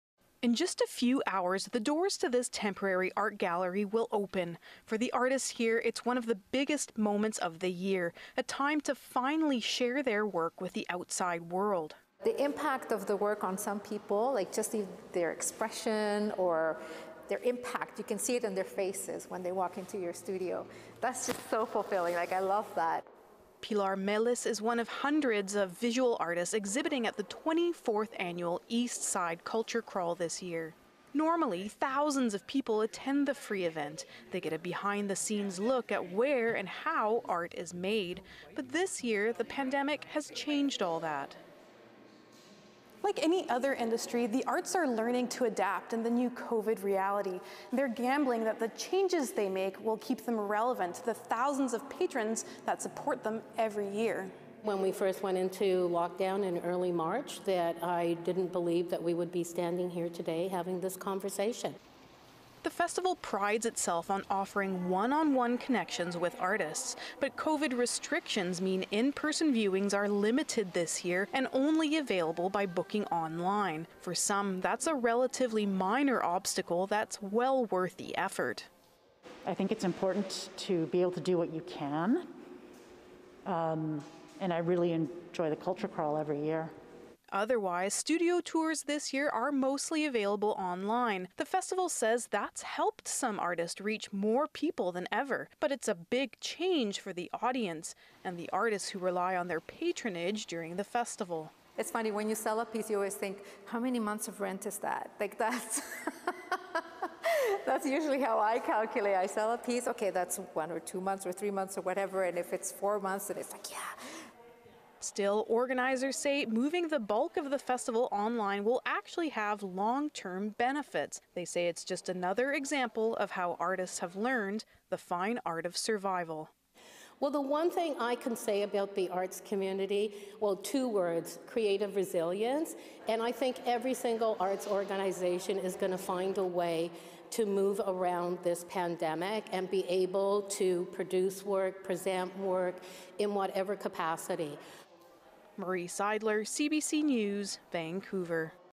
Eastside Culture Crawl goes online, and hopes its audience comes with it
Normally, thousands of people roam the streets of East Vancouver exploring artist studios as part of the Eastside Culture Crawl. But artists and patrons have had to adapt this year, by going mostly online and with limited in-person viewings booked online, spread over two weekends.

This is the first in a four-part series about artists, arts organizations and festival trying to survive during the pandemic.